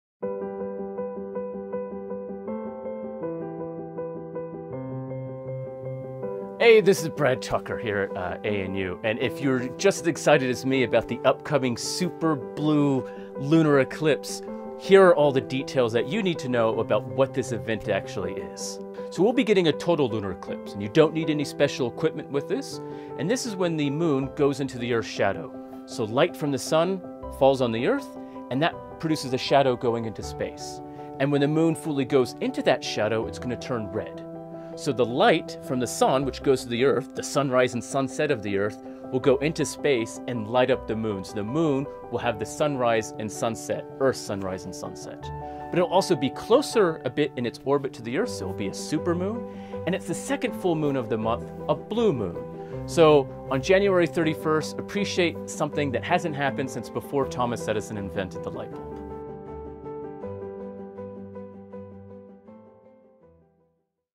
Super Red Blue Moon? What is a that?
ANU Astrophysicist Brad Tucker explains what a  super red blue moon is.
On the 31st January 2018 a special event took place .